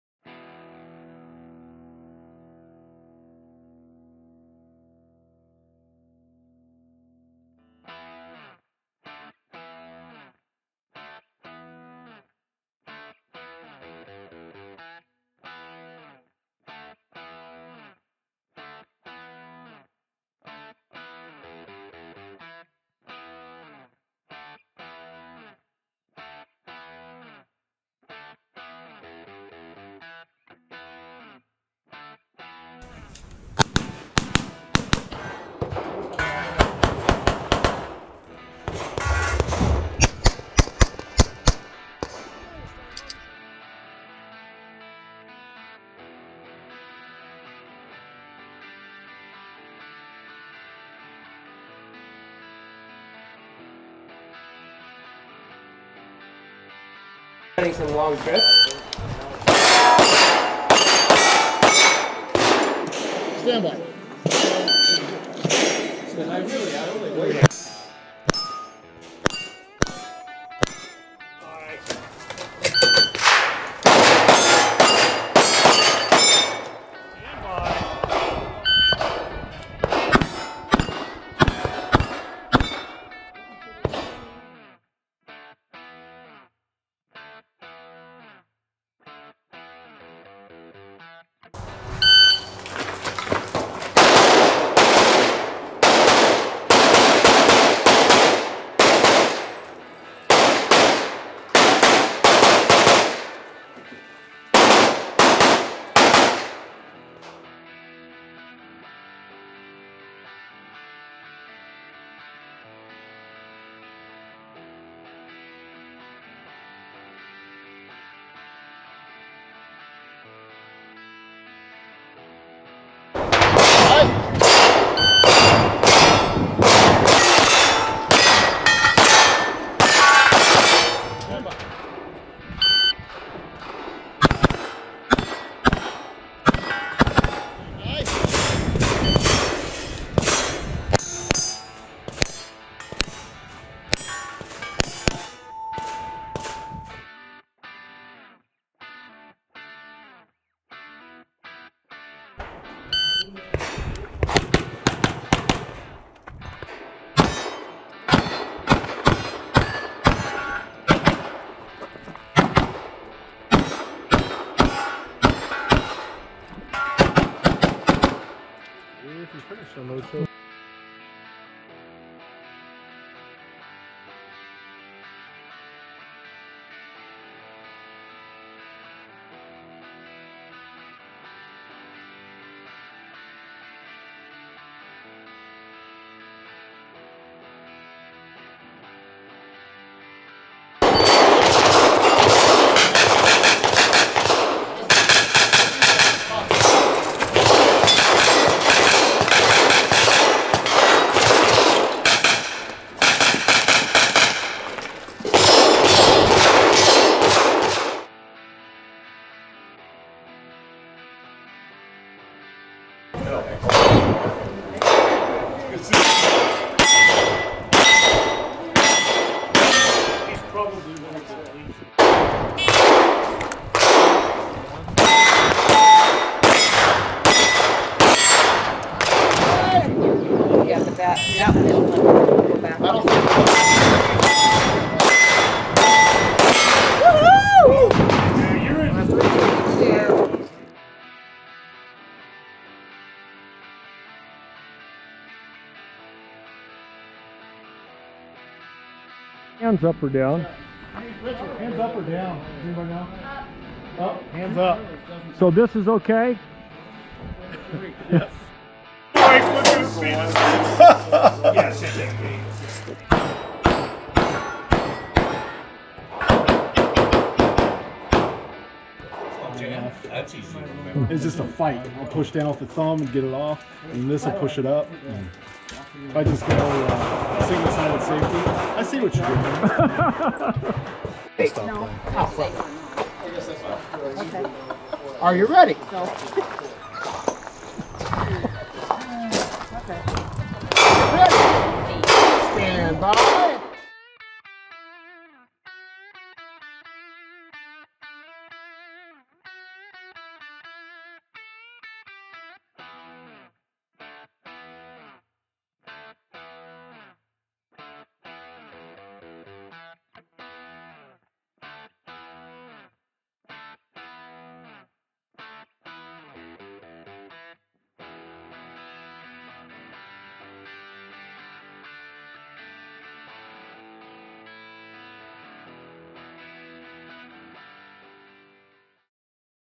ABC House 2019
This is the 2019 ABC House Match held at the Albany Rifle and Pistol Club in Albany Oregon.  As you will see this is a really fun match with six quick fun stages a great lunch and 120 competitors.  If you are looking to have a good time on one Saturday a year and believe in kids this is the one.
Thanks to Brian and Brian for all your hard work on this match, the Donations from Springer Precision, Bowers Group, Thor Targets and North West Self Defense Education.